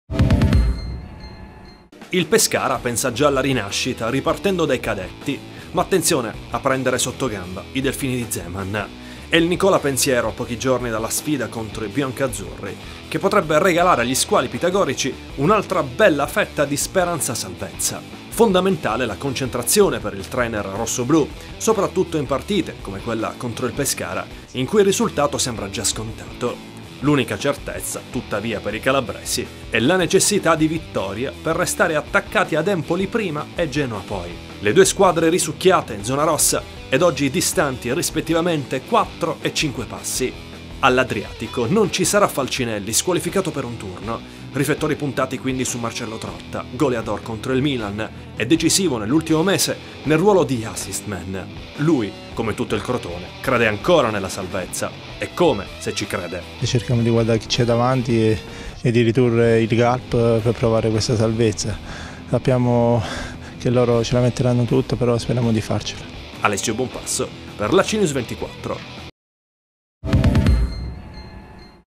Crotone, Trotta: "Salvezza? Non molliamo"
In Serie A il Crotone chiama a raccolta i suoi tifosi. Contro il Pescara chance concrete di avvicinare ancora di più Empoli e Genoa. Come ribadisce ai nostri microfoni l'attaccante rossoblu, Marcello Trotta.